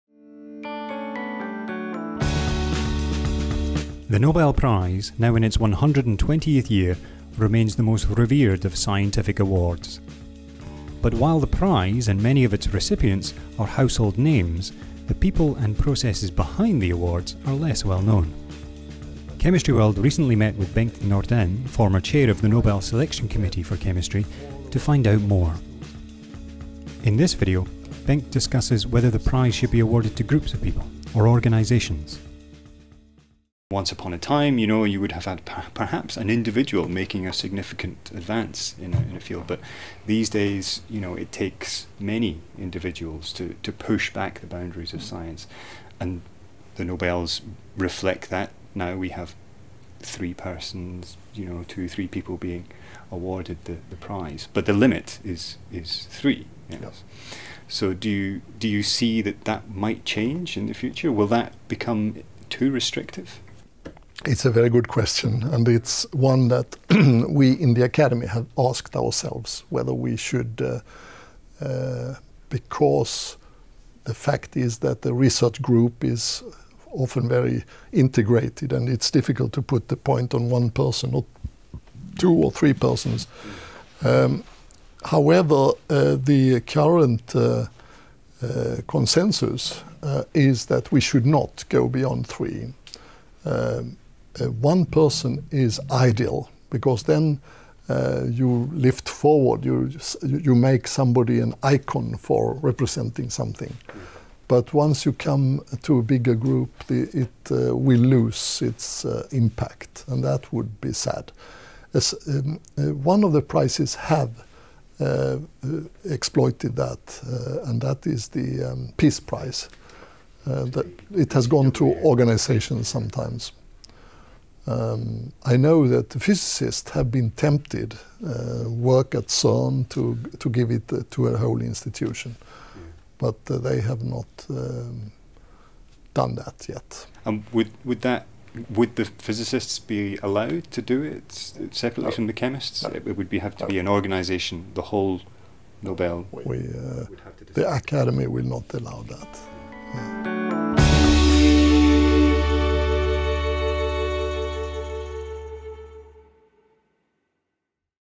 Behind the Nobels 5/7 - Will the chemistry Nobel go to a group or organisation?
Chemistry World spoke to Bengt Nordén, former chariman of the Nobel selection committee for chemistry. In this video, we ask Bengt if the Nobel committee has ever considered giving the chemistry prize to a research group or an organisation 5/7